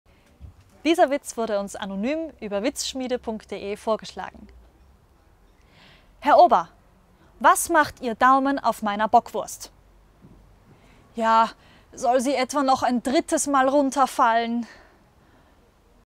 Die Bockwurst im Restaurant
"Herr Ober, was macht Ihr Daumen auf meiner Bockwurst?" - "Ja, soll sie etwa noch ein drittes Mal runterfallen?"

Vorgetragen von Otterienchen

Dieser Witz wurde vorgeschlagen von 'Pemu4'.